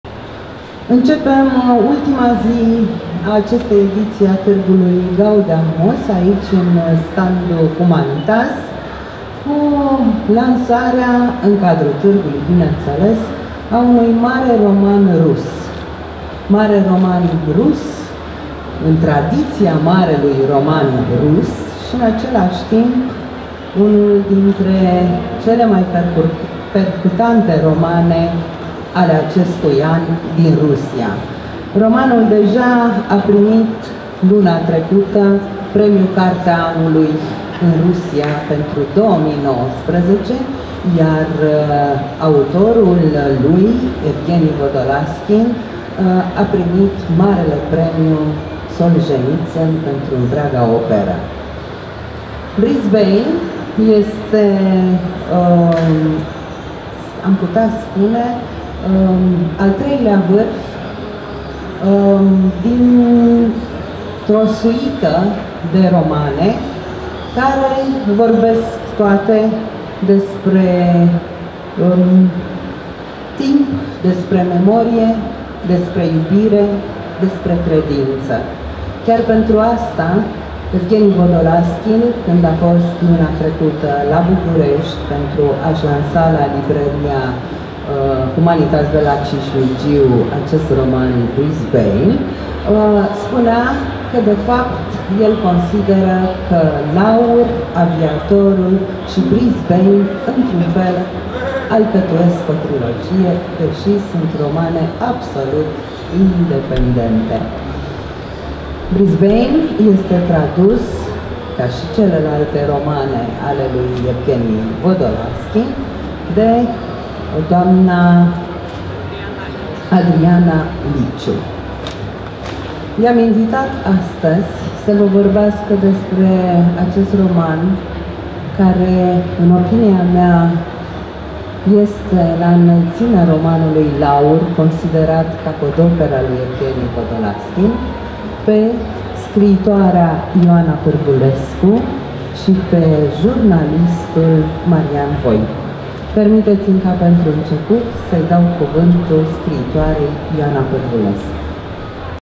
(01) Denisa Comănescu - Lansare de carte - "Brisbane" de Evgheni Vodolazkin (2019.11.24, Gaudeamus)
În data de 24 noiembrie 2019 a avut loc lansarea de carte "Brisbane" de Evgheni Vodolazkin în cadrul târgului Gaudeamus 2019.

Vorbitori: Ioana Pârvulescu, Marian Voicu, Denisa Comănescu.